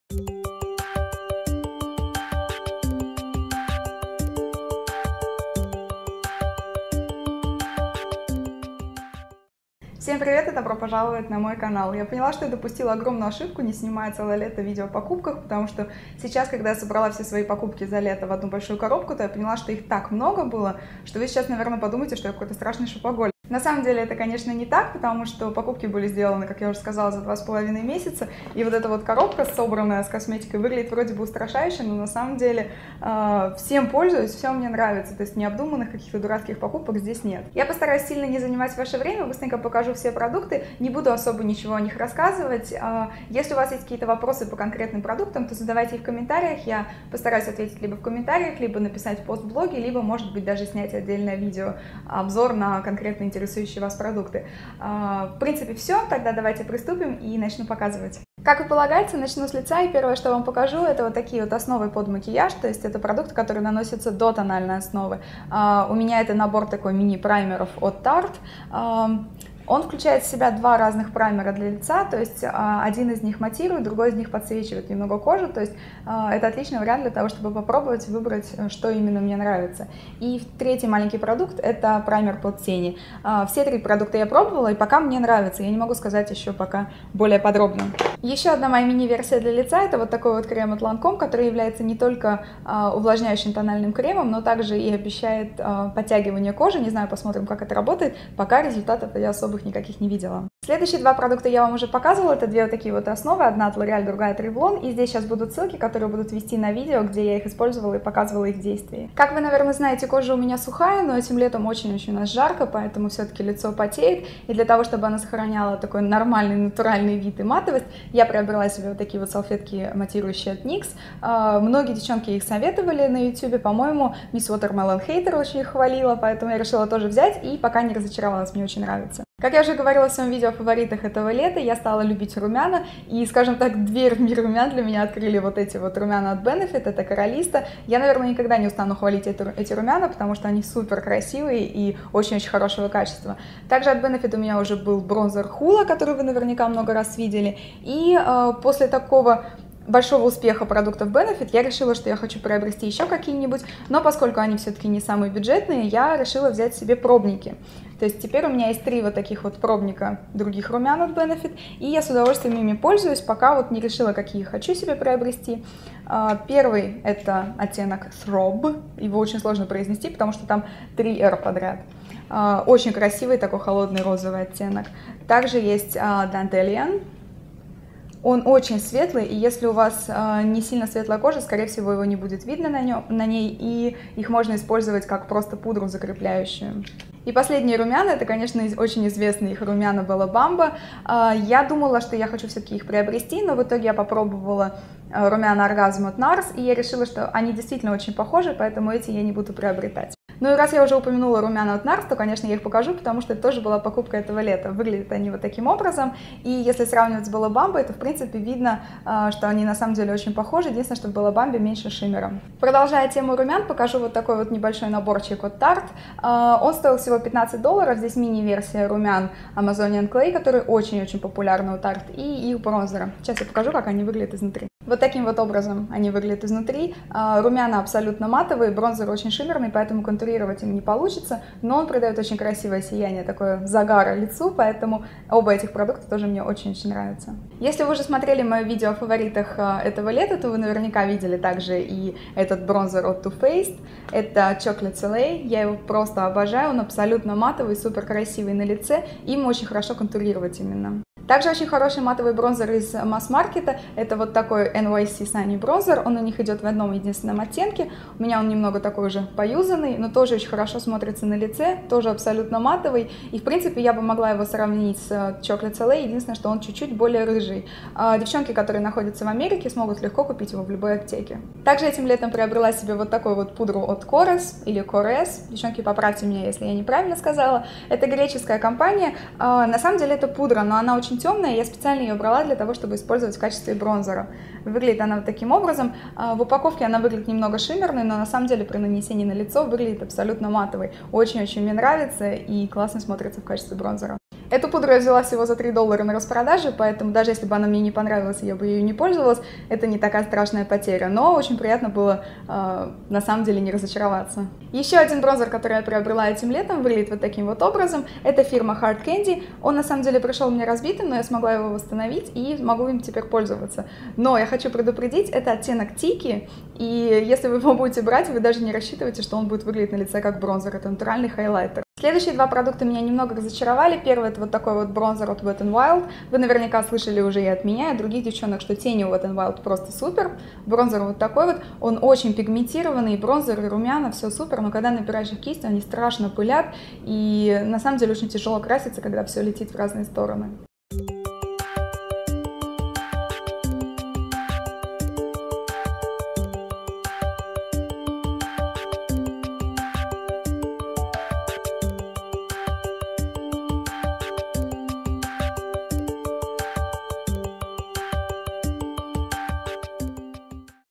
Покупки косметики за лето. Часть 1 / отзывы о косметике
Где еще меня можно найти:
В видео показаны следующие продукты:
Набор мини-праймеров для лица и глаз от Tarte
Увлажняющий тональный крем Lancome Renergie-Eclat Multi-lift в оттенке 2
Тональная основа L'oreal True Match Lumi в оттенке creamy natural
Тональная основа Revlon Colorstay Whipped в оттенке 200 Sand Beige
Матирующие салфетки от NYX
Румяна Benefit Coralista
Румяна Benefit Thrrrob
Румяна Benefit Dandelion
Румяна Benefit Bella Bamba
Румяна и бронзер NARS orgasm and laguna blush and bronzing duo
Румяна Tarte amazonian clay в оттенке Dollface
Бронезер Tarte в оттенке Park ave princess
Too Faced Chocolaté soleil bronzer
Бронзер NYC sunny
Пудра Korres в оттенке MVP7
Бронзер Hard Candy в оттенке Tiki
Бронзер+румяна Wet'n'wild в оттенке Me Shell My Belle
Обязательно смотрите ответы на ЧаВо. Они постоянно обновляются и дополняются.
Результаты конкурсов мне уже известны, скоро объявлю их у себя в блоге :)